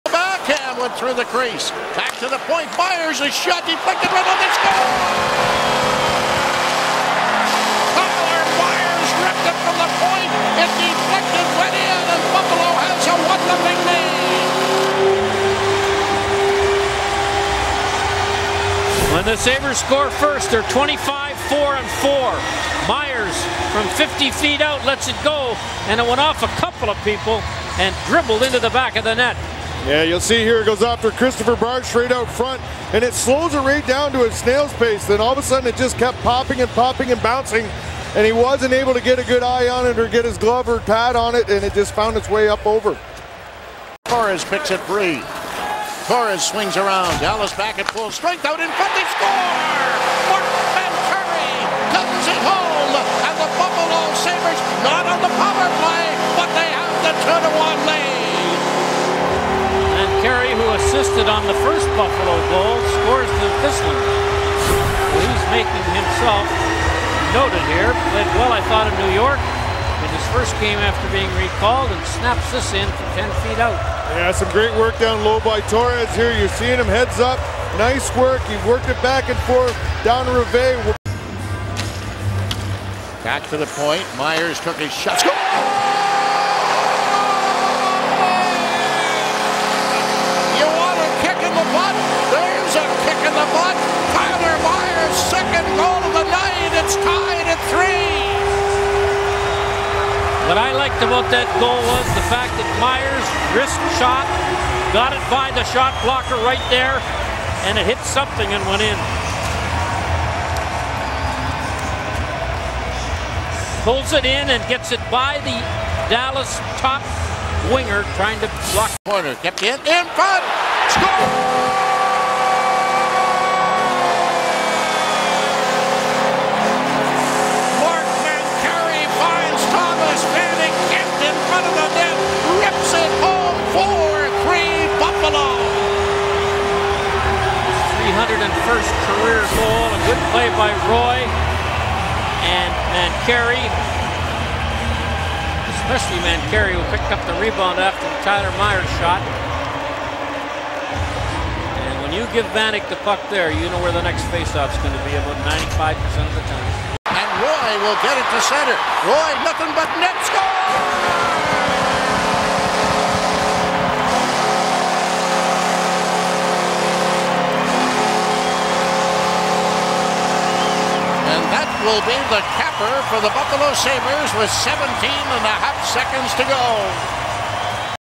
Sabres Goals Vs Dallas 03/10/10
The Sabres score 3 goals in a wild first period, once in the 2nd, and an empty netter in the 3rd period to secure a 5-3 victory, at home, over the Stars. March 10, 2010